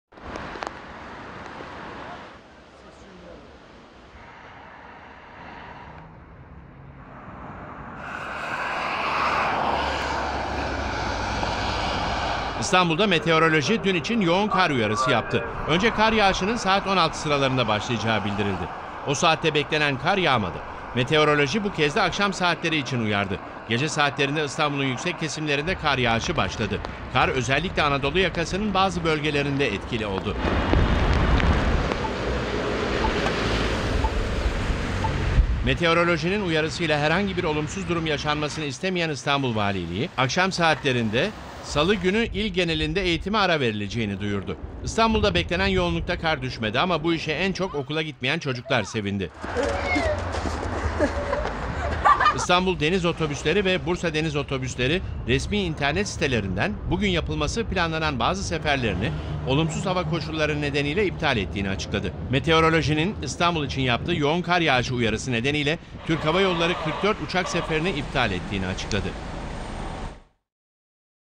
KARDAN ÖNCE TATİL GELDİ!
Türkiye, Sibirya'dan gelen dondurucu havanın etkisi altına girdi. Beklenen kar, beklendiği saatinde İstanbul'a yağmadı. Ancak valilik uyarıları  dikkate alarak okulları tatil etti. İstanbul'a 'randevuyla gelen karın", beklenti ve tahminlerin altında kalması ise sosyal medyada espiri konusu oldu.